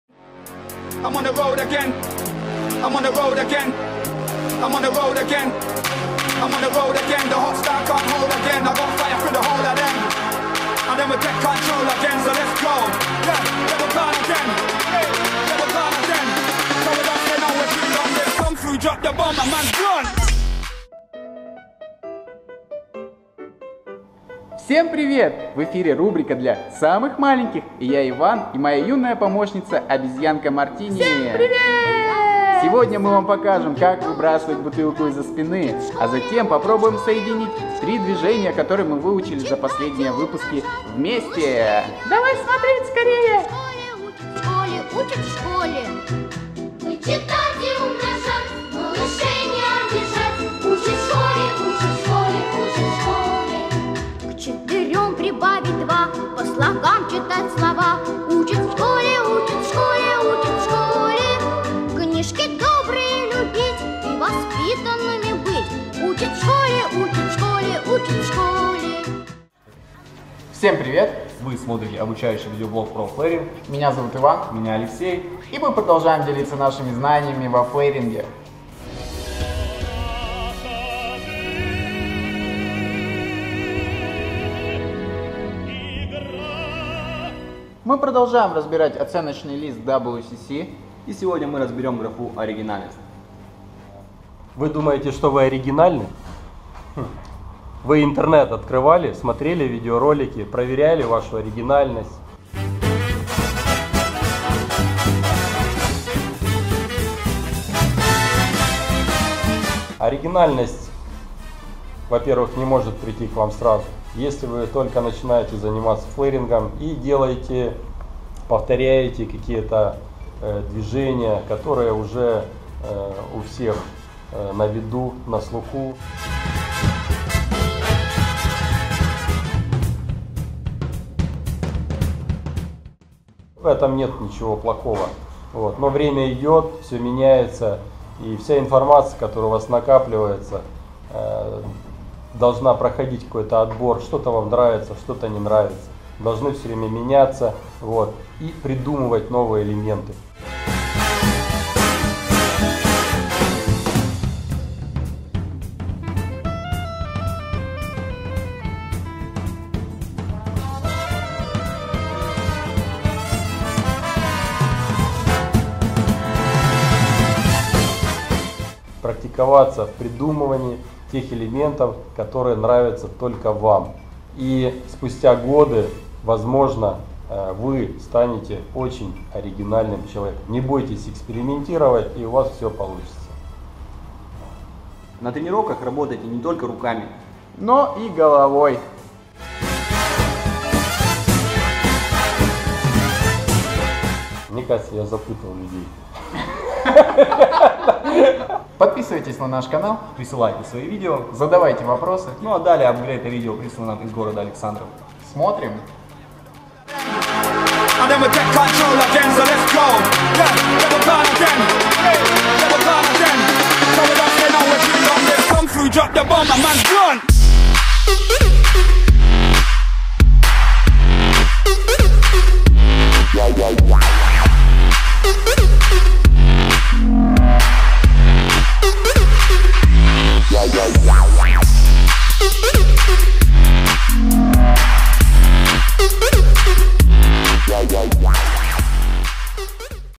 Флейринг обучение | Школа барменов | 18
Обучающий Видео-блог с уроками флейринга от Bartrender Tv!
В комментариях кидайте видео с вашими связками "на прокачку"!